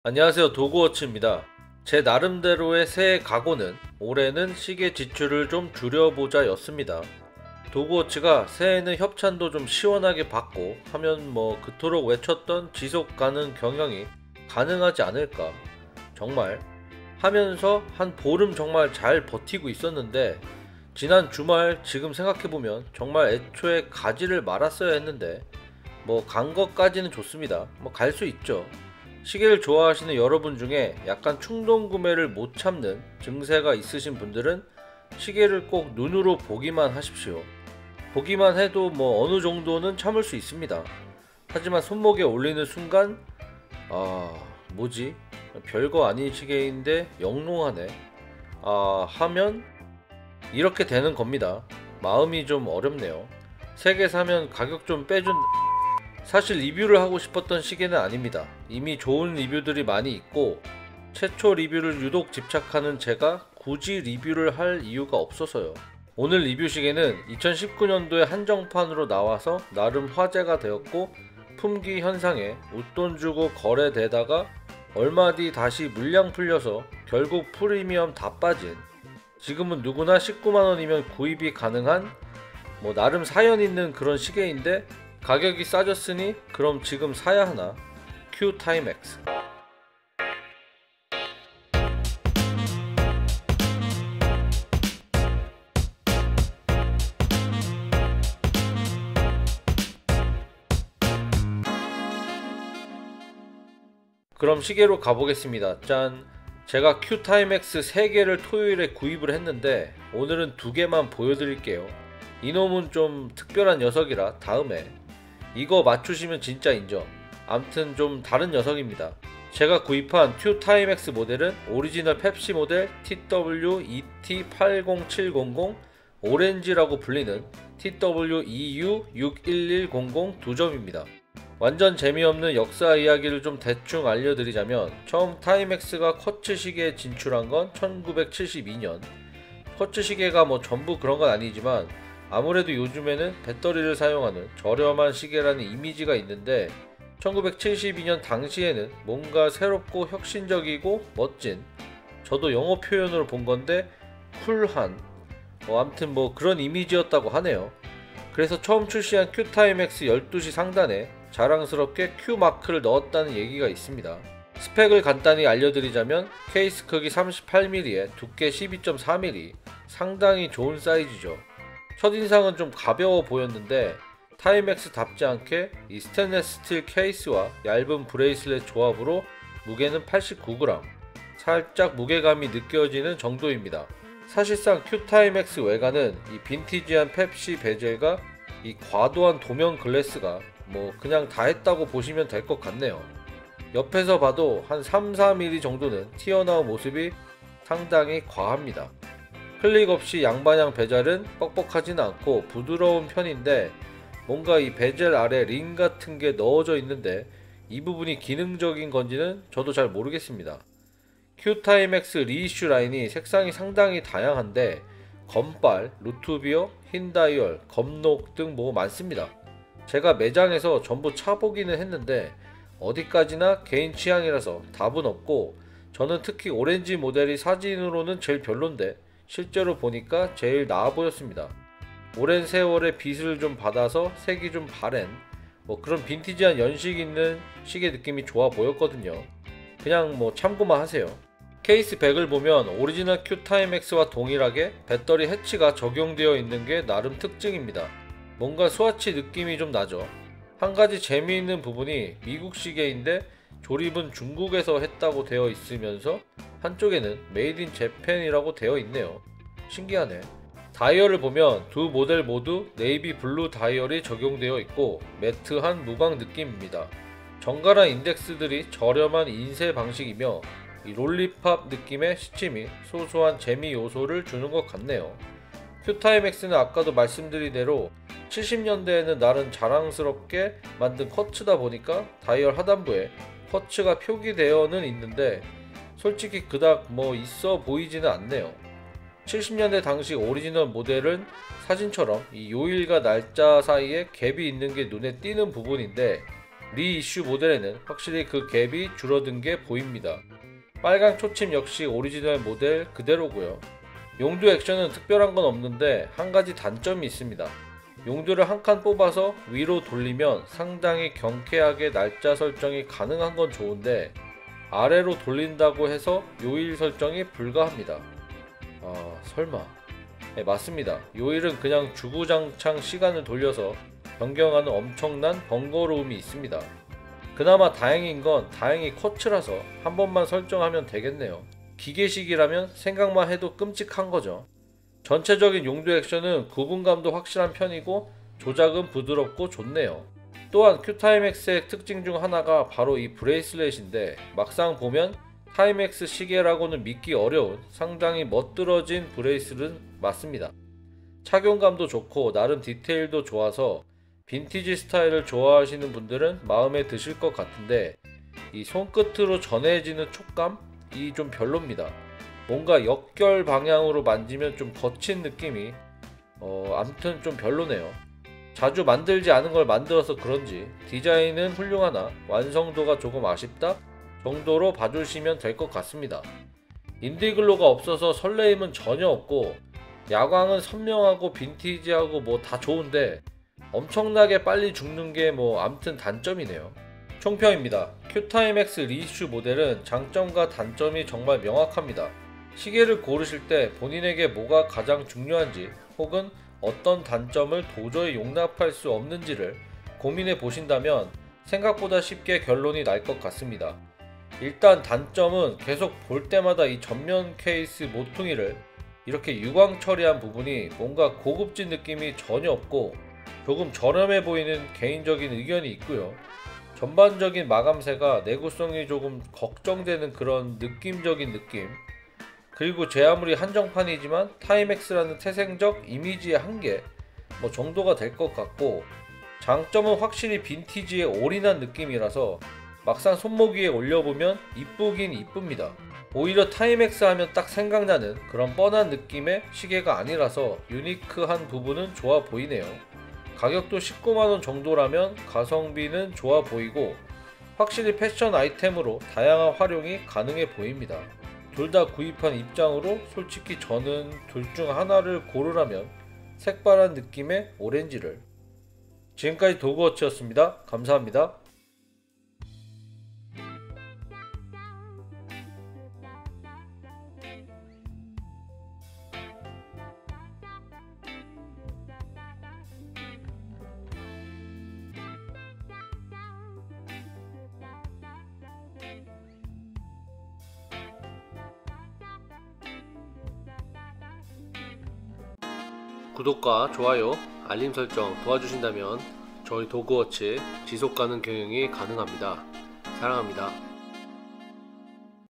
[도그워치 리뷰#50] 지금사면 싸다? 한정판 큐타이맥스 리뷰 Q Timex 가격이 올랐던 최초의 타이맥스??
2019년도에 출시한 사연?있는 시계 큐타이맥스 리뷰입니다.
출시 직후 50만원까지 올랐던가? 지금은 저렴하게 구입이 가능한데..
과연 살만한 시계일까요?